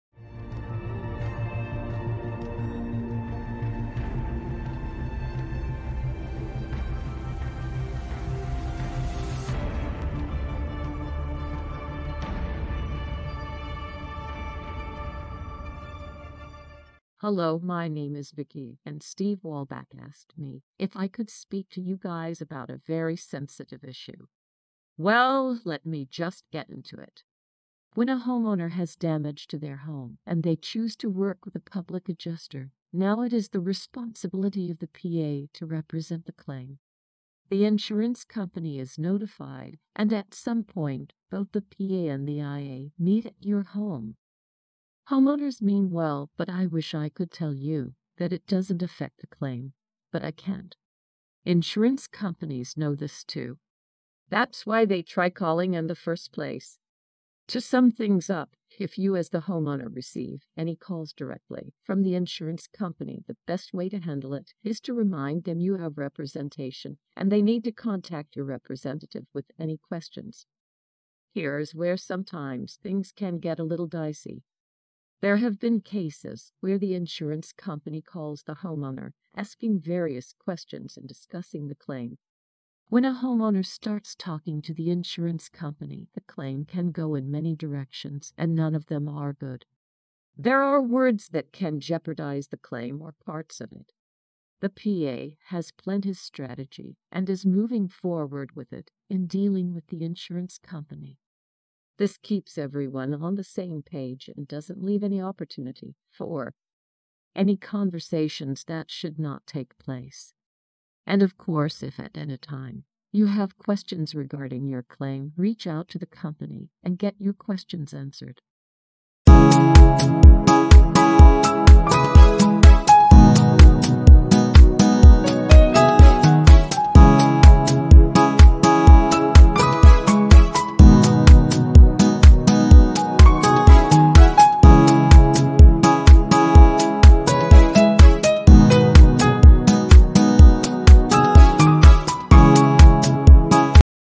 Working With A Public Adjuster, No Worries, Right 2
Claims can be denied or they get paid, don't you want to
stack the cards in your favor? Having the right policy is
so important.
Steve Wolbach Your Friendly Public Adjuster